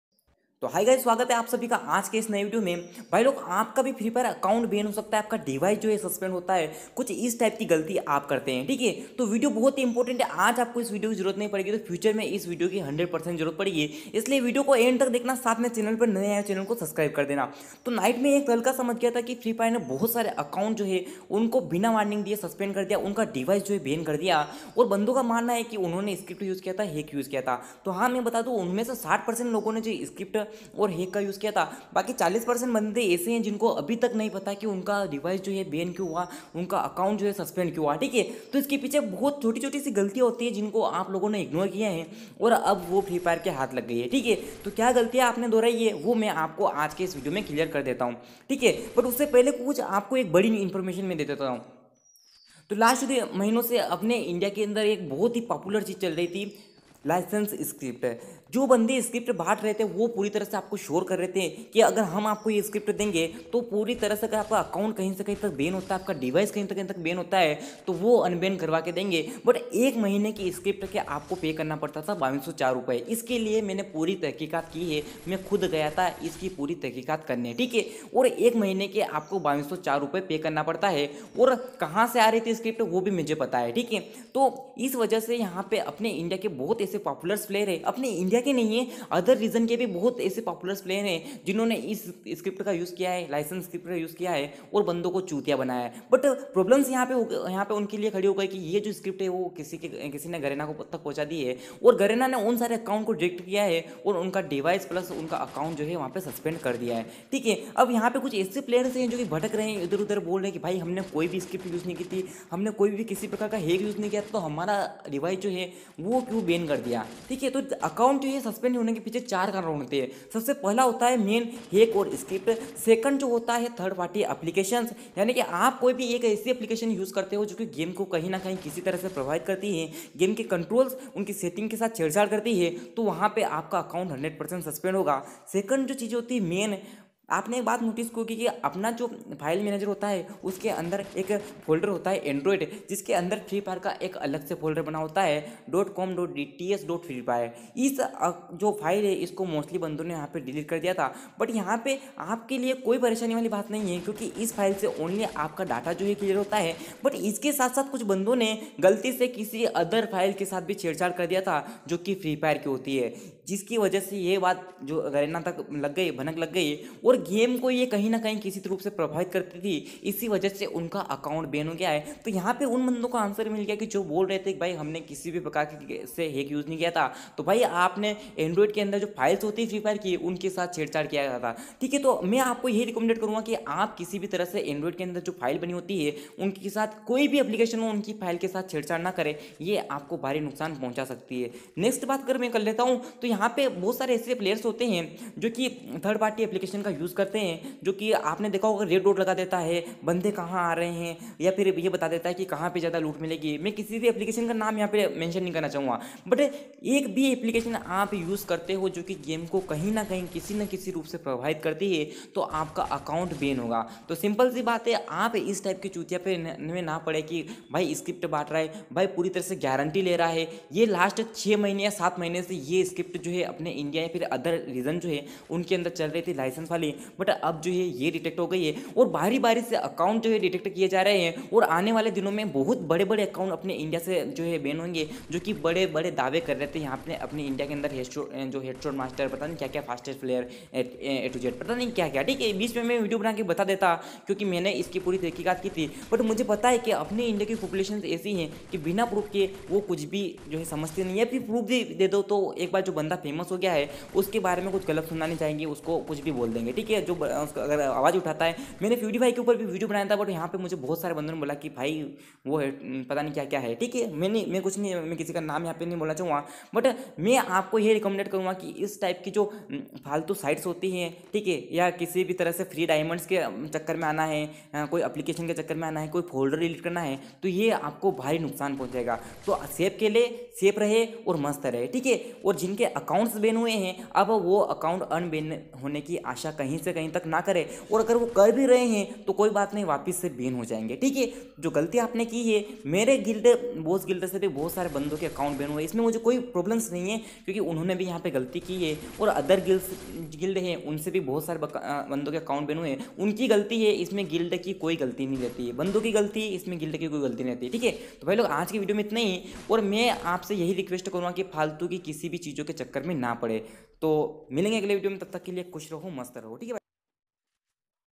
Free Fire account ban बिना hack use किये भी हो सकता है,free fire account suspend ये गलती मत करना ?
-why garena suspend their account
-Account /device suspend without use hack ? how🤔
-4 reason  to suspend your free fire account

device---
      My free fire id. -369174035f
      Guild - Boss
      Device - poco F1
      Ram/Rom. (6/64)